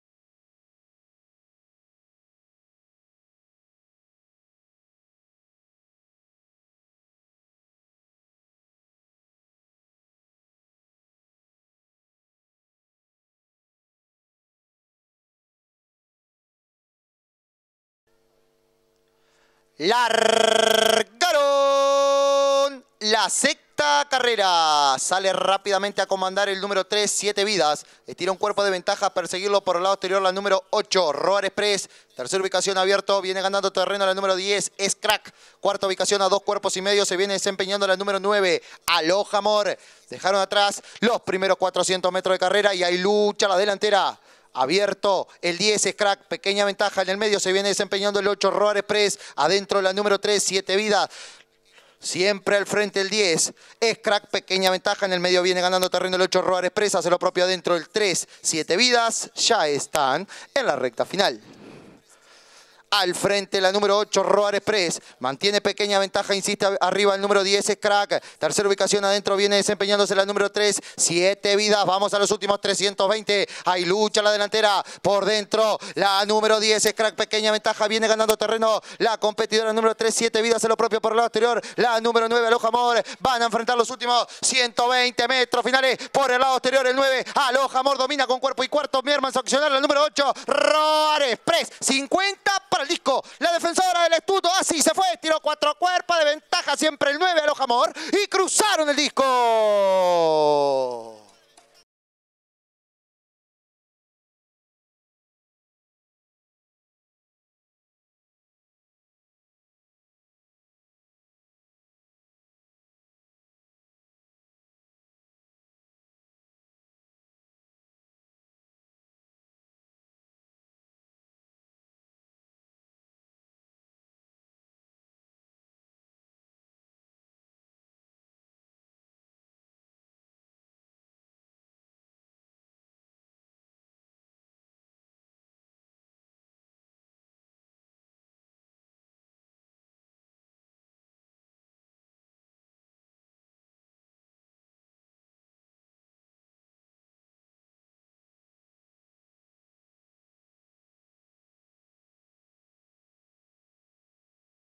17/10/2015 – Hipodromo Colonia – Carr 6 – TINTO CHILENO
Carrera 6 realizada el dia 17/10/2015 por el premio TINTO CHILENO en el Hipodromo Colonia